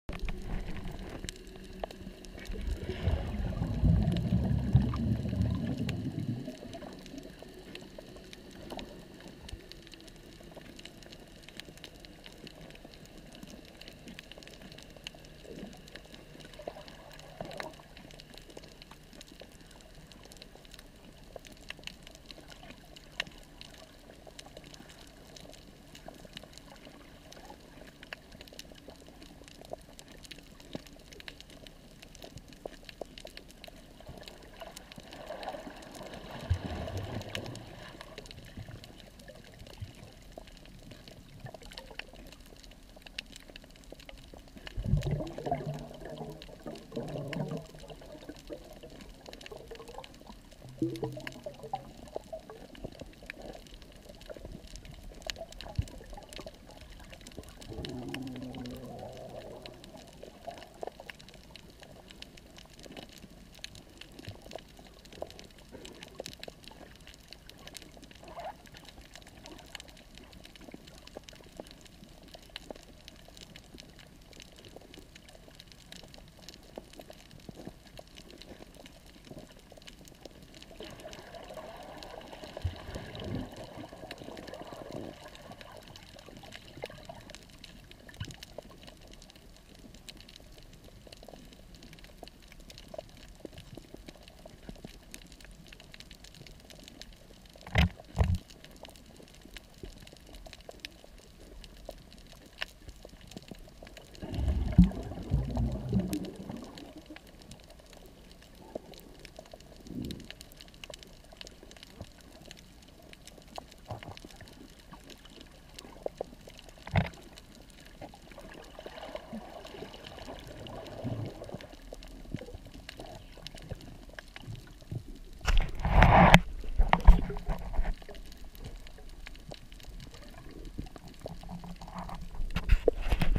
Gab Gab Beach Guam Snorkeling
Snorkeling in the Amazing waters of Guam.Gab Gab beach has a lot of Anemone Fish.  The coolest little fish you will see.